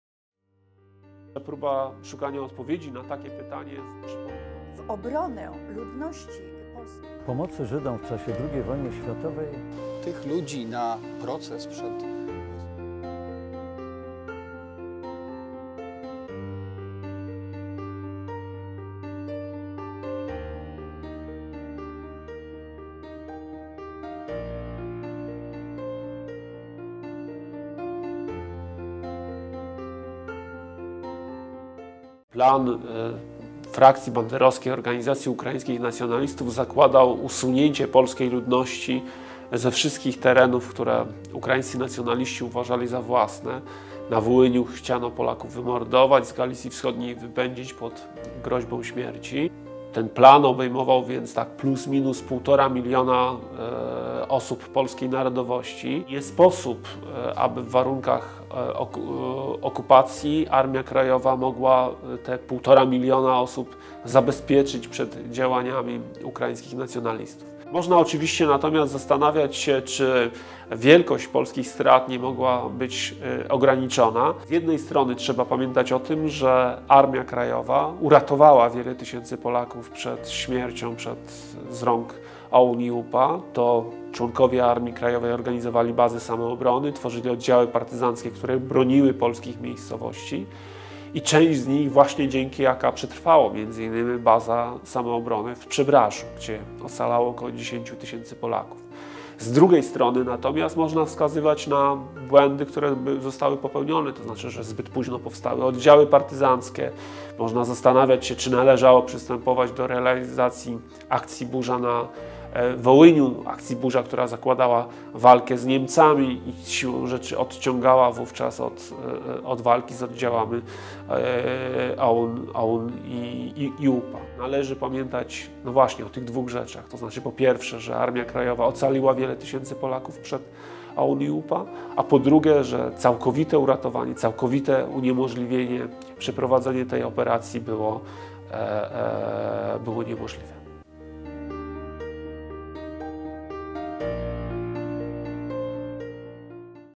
IPN TV - Pytania o Polskie Państwo Podziemne - dr hab. Grzegorz Motyka
dr hab. Grzegorz Motyka - Przewodniczący Rady IPN, w ramach projektu pytania o Polskie Państwo Podziemne, odpowiada na zagadnienie:

Czy Armia Krajowa mogła powstrzymać ukraińskich nacjonalistów przed wymordowaniem około 100 tysięcy Polaków?